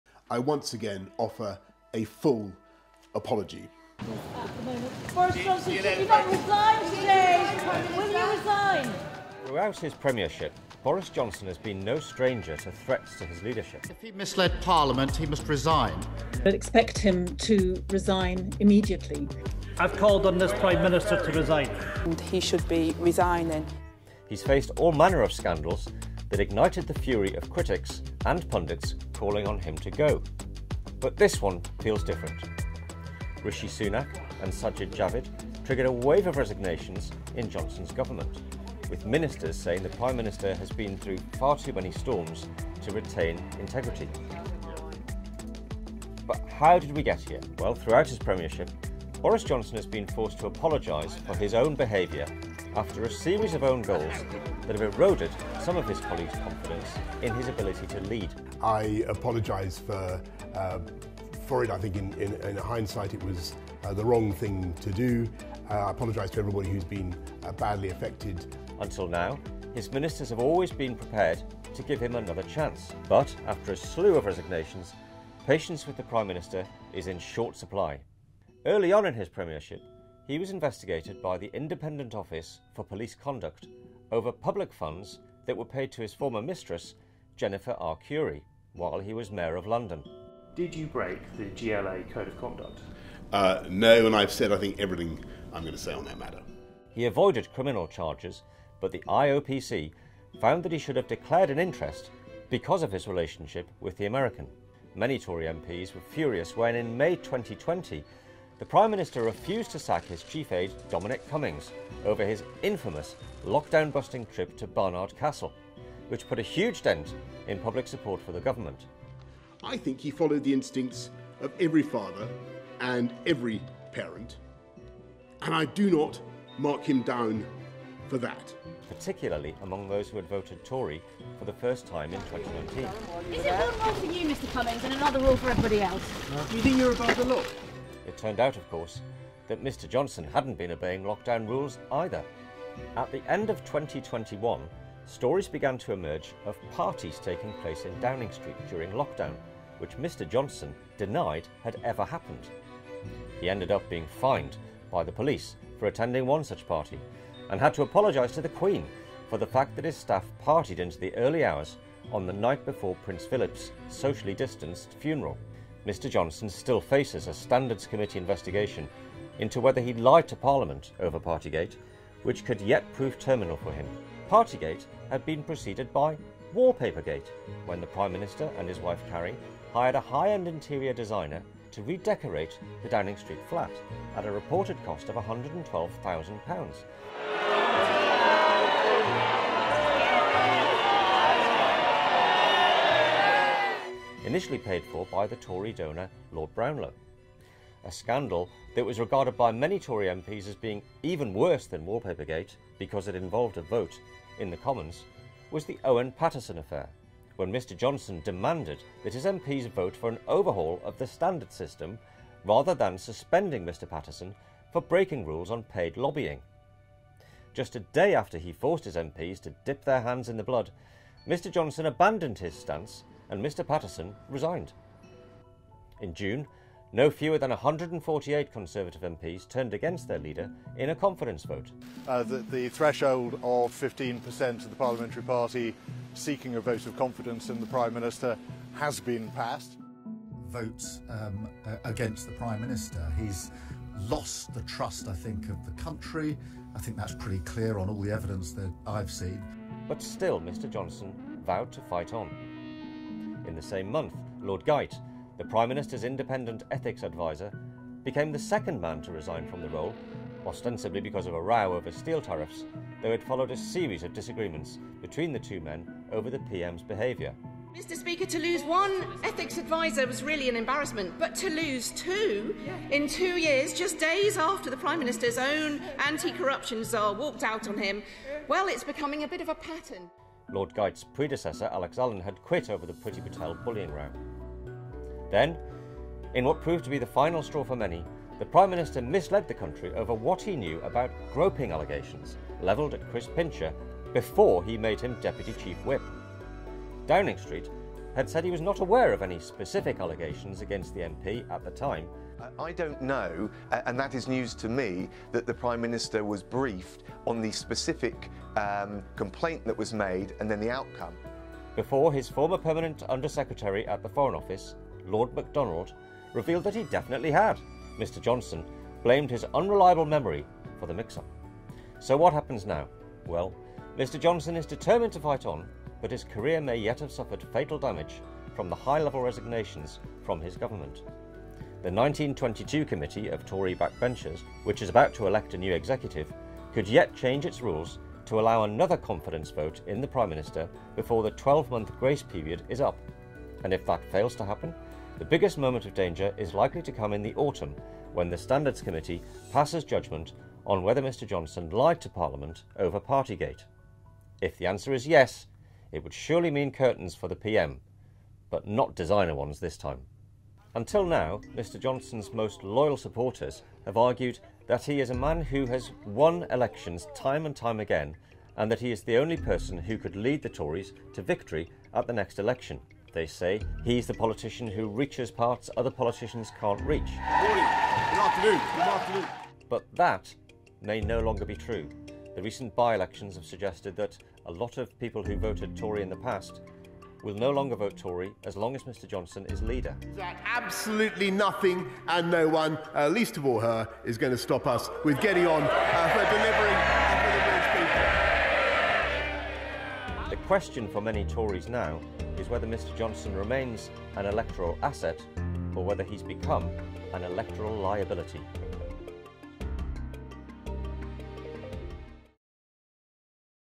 Where it all went wrong for Boris Johnson
Boris Johnson has suffered the most ministerial resignations in 24 hours since 1932, as his premiership plunged to its lowest point yet.

Since Tuesday evening 15 ministers have resigned as the fallout from Downing Street's handling of the Chris Pincher affair continues to grow. More than 30 Conservative MPs have quit their positions in total so far.